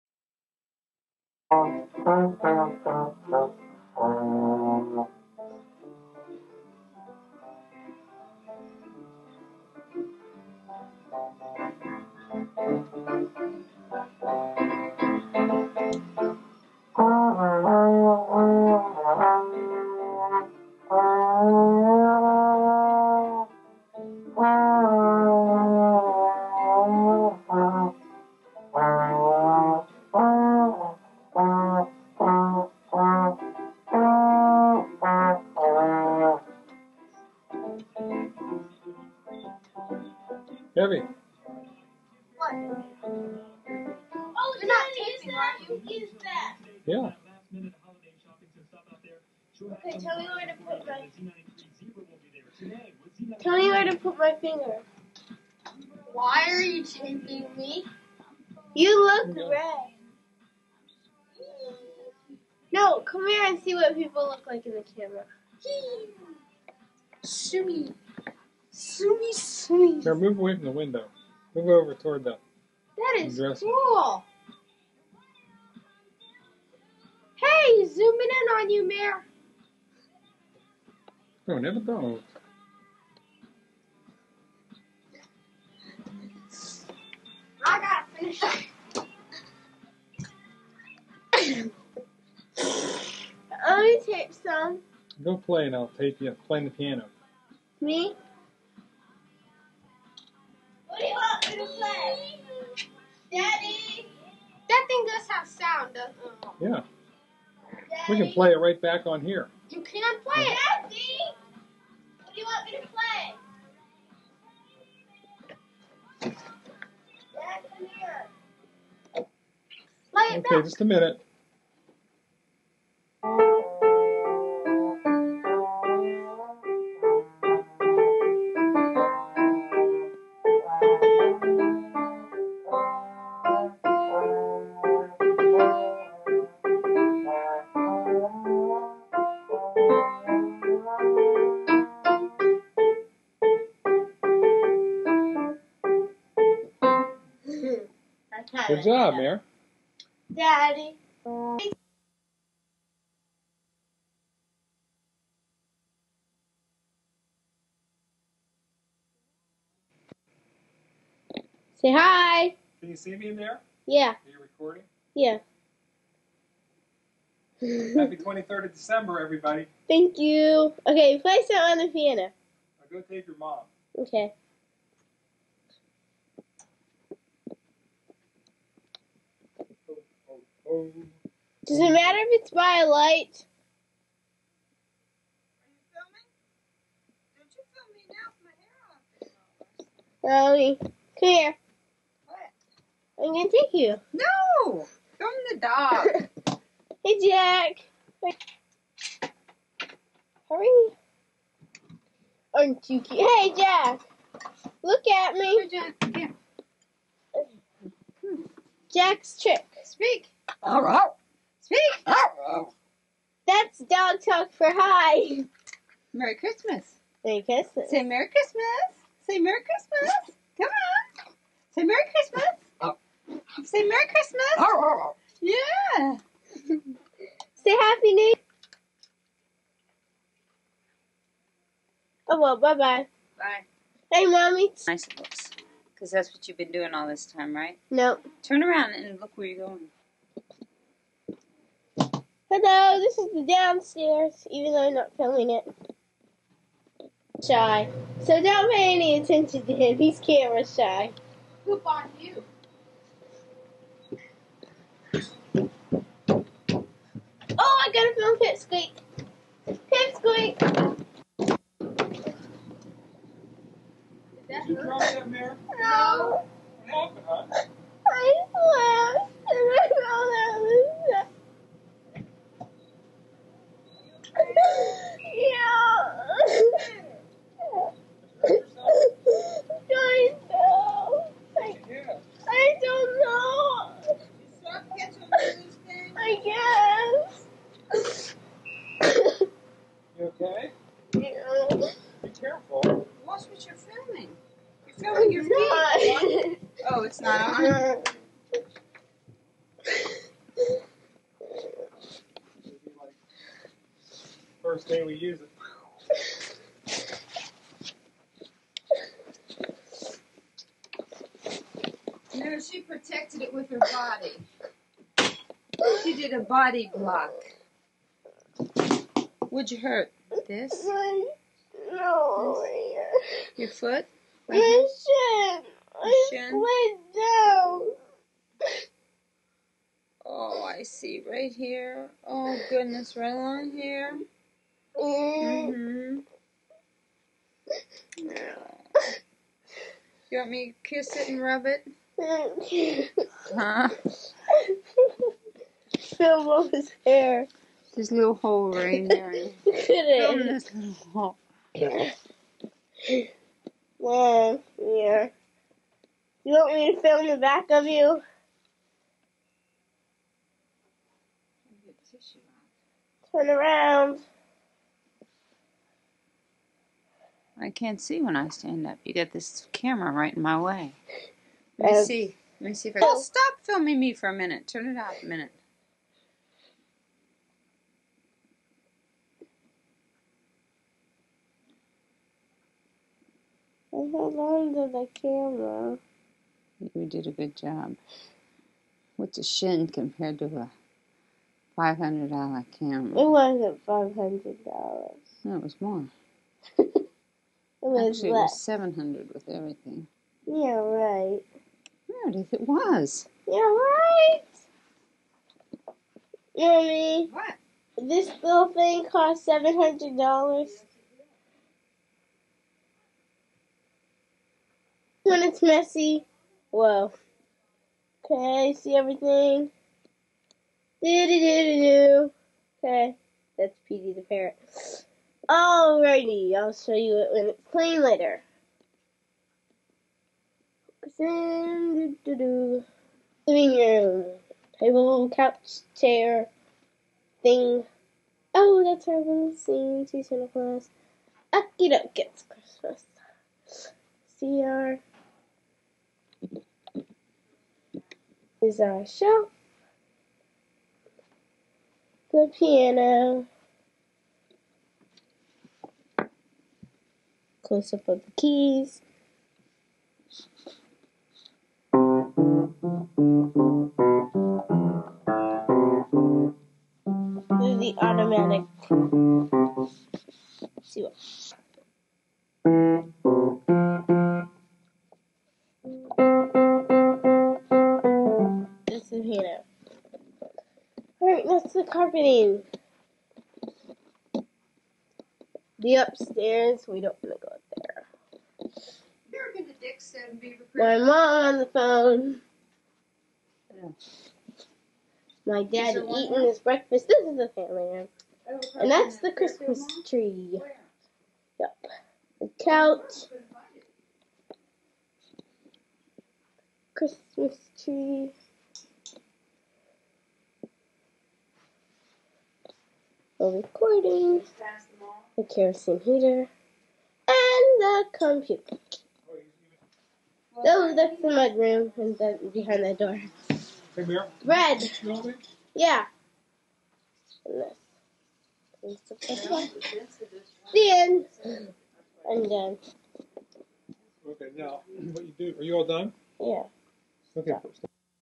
MTV Cribs: Brantford Edition • Dec 23, 1995
Meredith gives a tour of the house and then falls down the stairs. Evan plays trombone. Jack speaks!

FEATURING:
Evan
Meredith
Ben
Christa